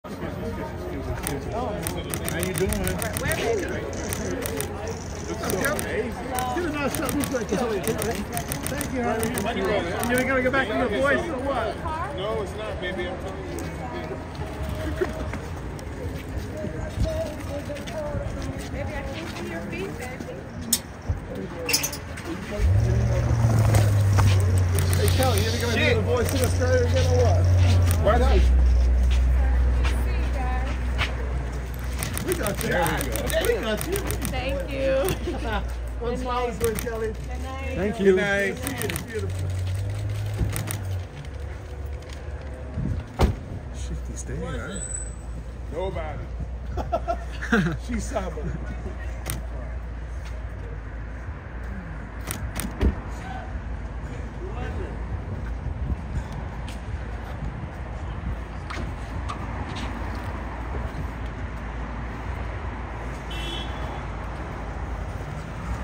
Kelly Rowland and Tim Weatherspoon dine out for a romantic dinner date for two in Santa Monica!
Kelly Rowland and husband Tim Weatherspoon walk hand in hand as they exit Italian restaurant Giorgio Baldi after having dinner in Santa Monica.